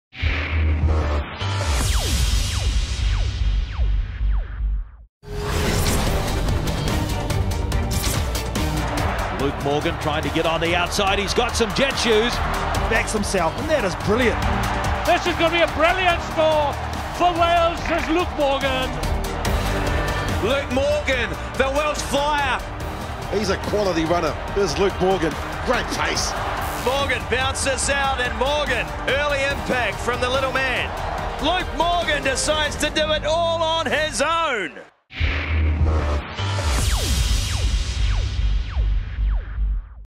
Olympics OneToWatch Luke Morgan
'Ones to Watch' – fast-paced edits of rugby star Luke Morgan in action in preparation for the Rio 2016 Olympics.